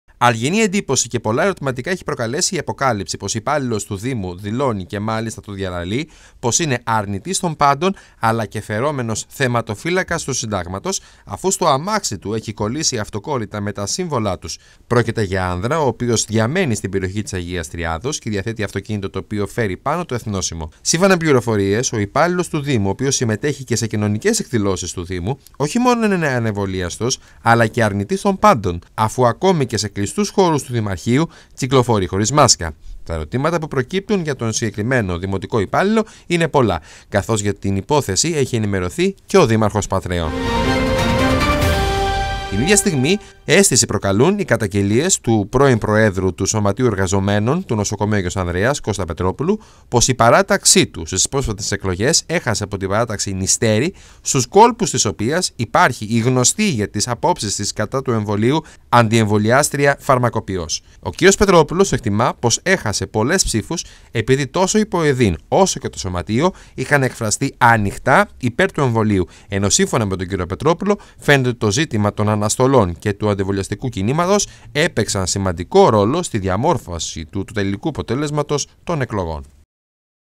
Υπάρχουν και στην Πάτρα "Θεματοφύλακες του Συντάγματος"
Θεματοφύλακες του Συντάγματος ως φαίνεται υπάρχουν και στην Πάτρα, ενώ ακόμη και στις συνδικαλιστικές εκλογές είχαν έντονο αποτύπωμα.